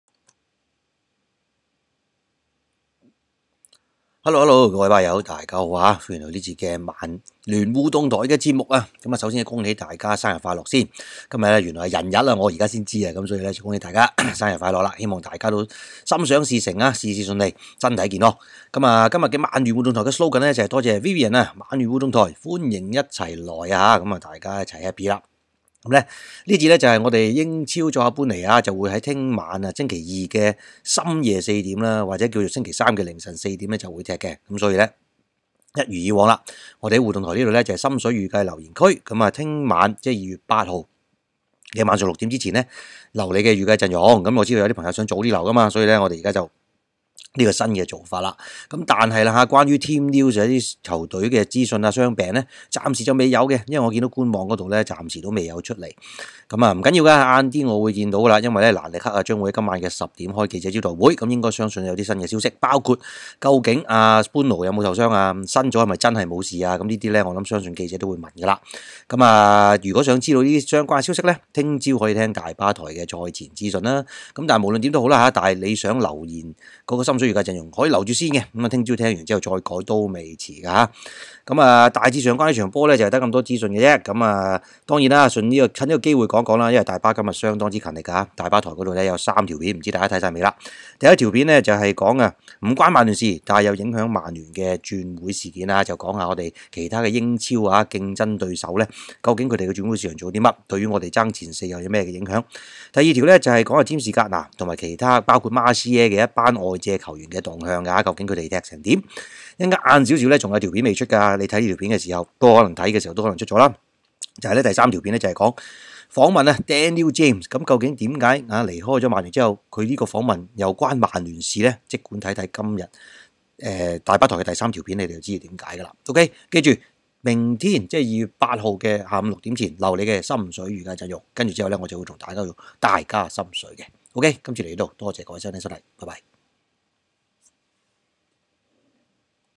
英超曼聯作客般尼 [24小時心水預計陣容留言區] ***大家記得6點前(2/8)留言心水預計陣容
曼聯互動台